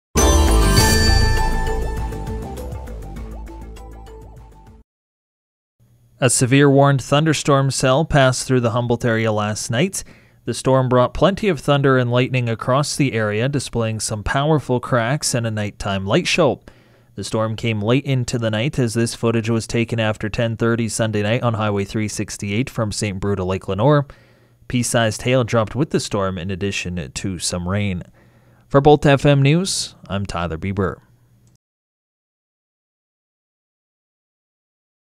July 30 Storm
A severe thunderstorm passed through the Humboldt area on July 30. The storm brought lightning, thunder, rain and pea sized hail.